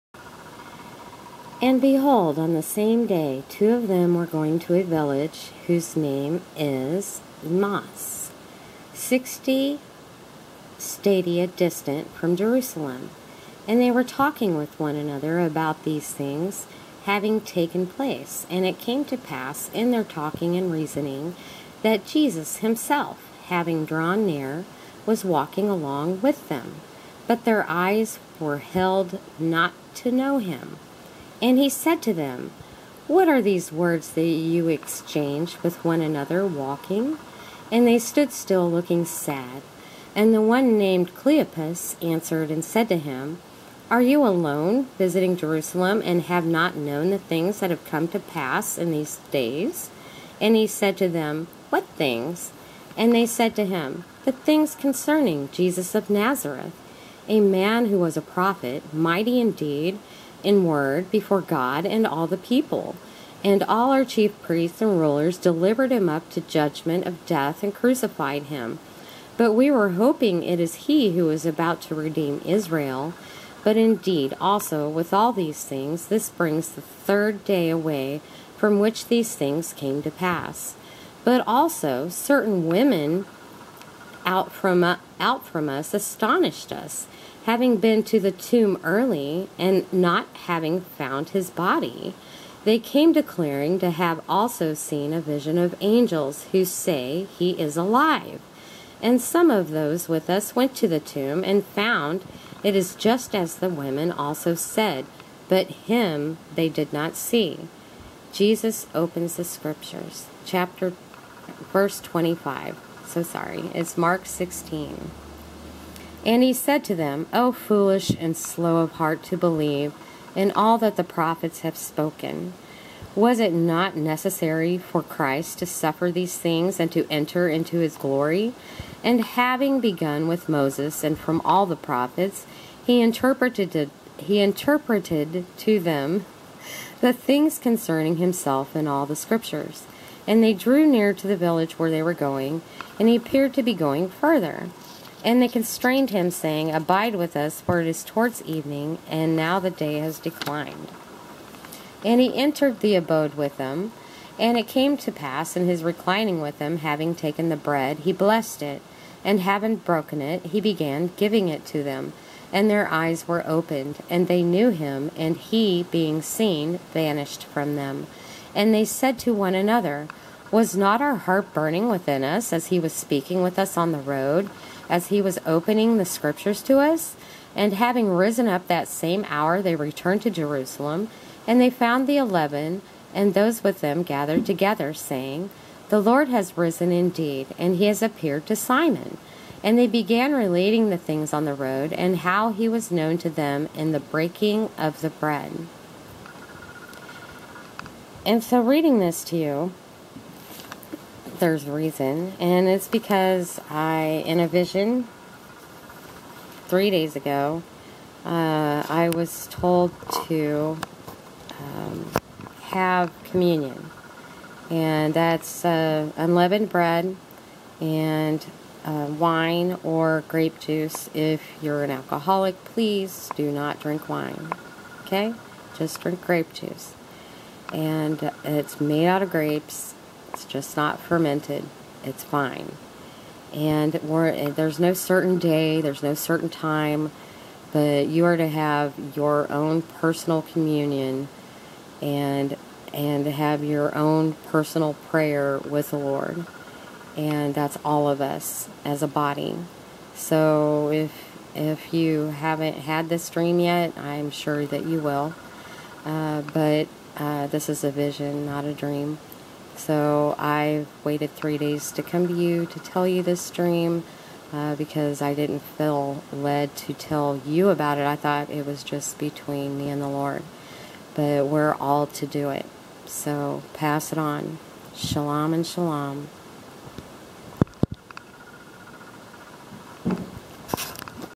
Breaking Bread /Communion
I was shown in a vison, 3 days ago, having communion by myself, breaking bread, (it was a cracker though) and sipping grape juice. In deep prayer with the Lord. Then this morning I was led to look up Mark 16-13-35.